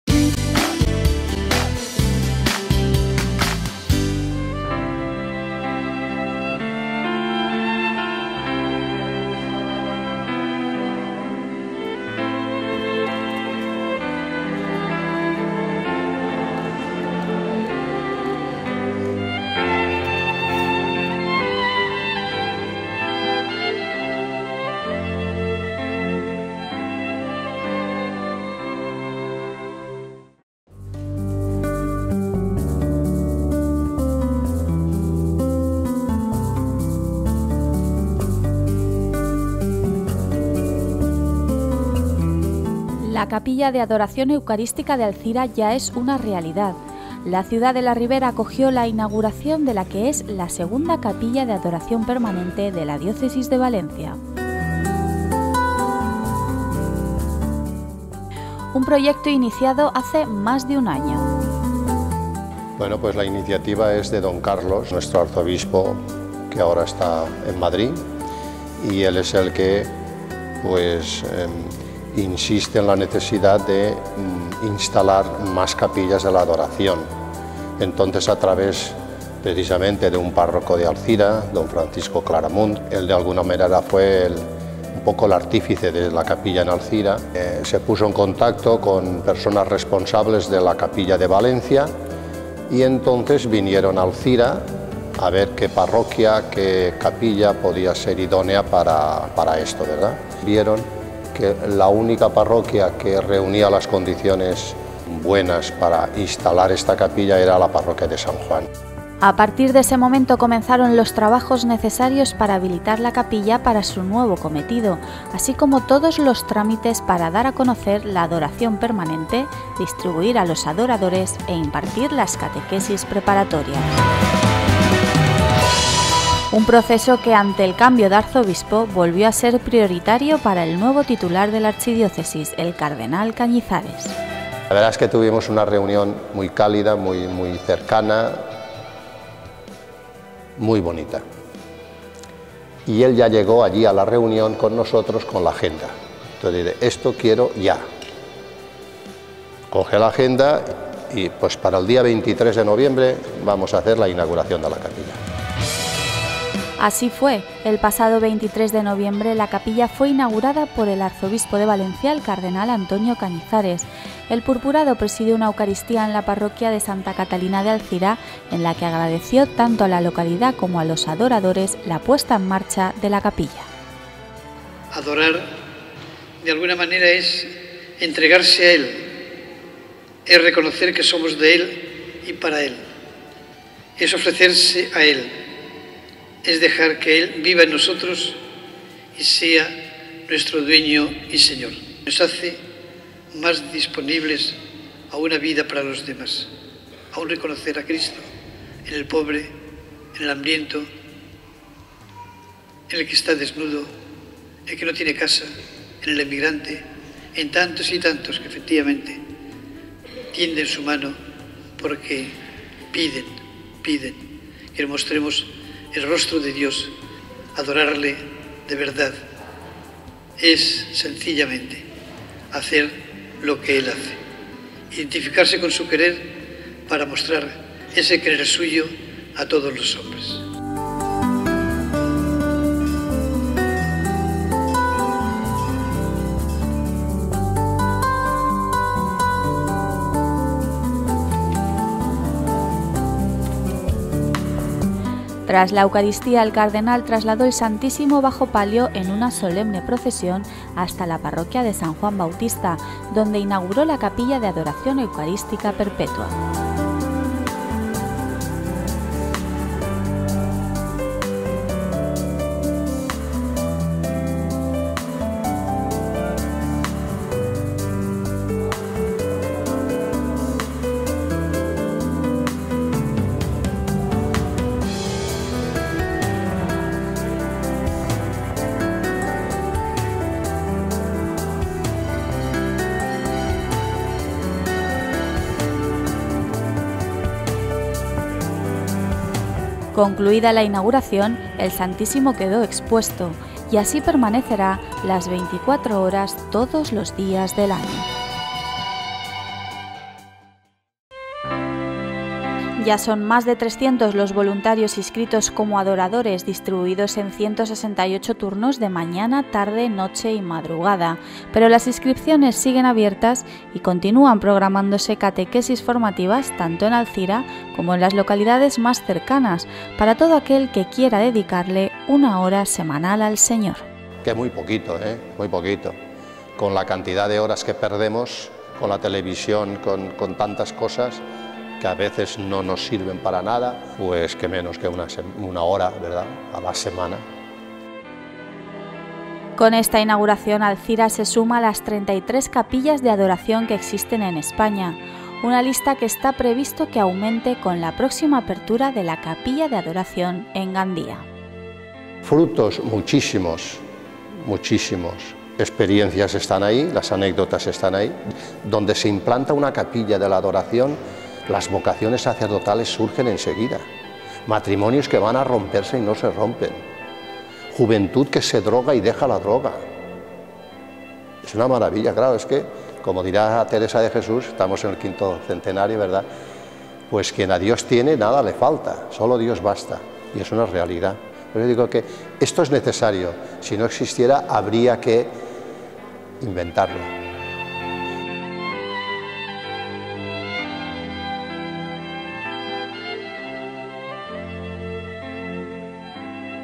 Capilla de Adoración Eucarística de Alzira
El cardenal Cañizares inauguró la capilla de Adoración Eucarística de Alzira, la segunda de toda la archidiócesis de Valencia.